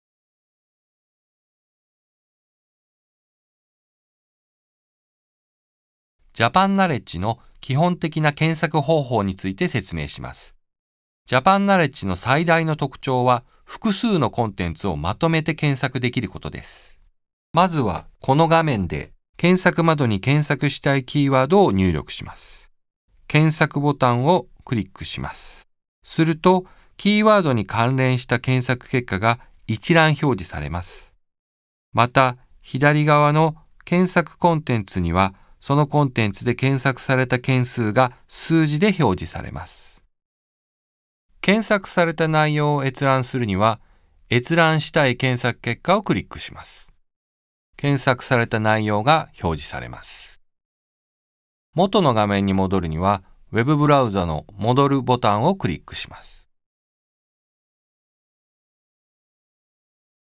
ジャパンナレッジの使い方 3. 基本検索
学術データベースJapanKnowledge(ジャパンナレッジ／ＪＫ)の基本的な検索方法について説明します。最大の特徴は、複数のコンテンツをまとめて検索できることです。「検索」ボタンをクリックします。すると、キーワードに関連した検索結果が一覧表示されます。